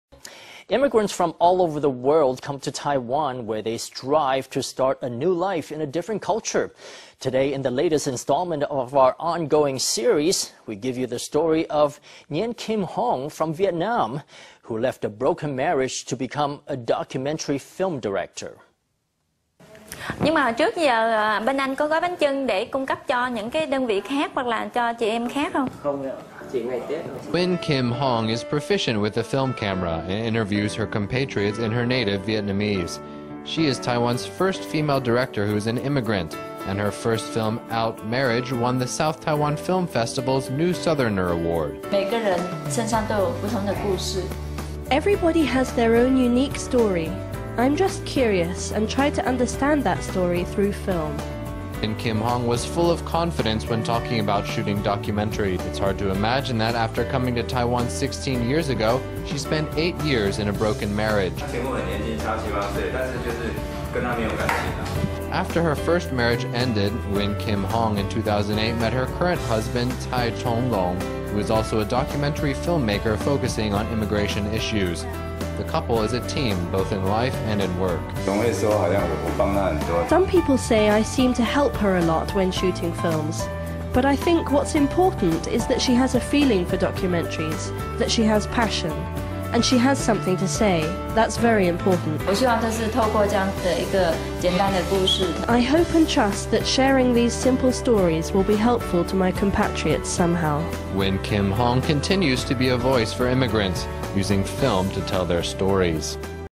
Nguyen Kim Hong from Vietnamtells the stories of her compatriots through film
Immigrants from all over the world come to Taiwan, where they struggle to start a new life in a different place. Today, in the latest part of our ongoing series we''d like to bring you the story of Nguyen Kim Hong from Vietnam, who left a broken marriage to become a documentary film director. Nguyen Kim Hongis proficient with a film camera, and interviews her compatriots in her native Vietnamese. She is Taiwan’s first female director who is an immigrant, and her first film ‘Out/Marriage’ won the South Taiwan Film Festival’s ‘New Southerner’ Award.Nguyen Kim HongVietnamese ImmigrantEverybody has their own unique story. I’m just curious, and try to understand that story through film.Nguyen Kim Hong was fullof confidence when talking about shooting documentaries. It’s hard to imagine that, after coming to Taiwan sixteen years ago, she spent eight years in a broken marriage.After her first marriage ended, Nguyen Kim Hong in 2008 met her current husband Tsai Chung-lung, who is also a documentary filmmakerfocusing on immigration issues. The couple is a team both in life and in work.Tsai Chung-lungHusband of Nguyen Kim HongSome people say I seem to help her a lot when shooting films. But I think what’s important is that she has a feeling for documentaries, that she has passion. And she has something to say – that’s very important.Nguyen Kim HongVietnamese ImmigrantI hope and trust that sharing these simple stories will be helpful to my compatriots somehow.Nguyen Kim Hong continues to be a voice for immigrants, using film to tell their stories.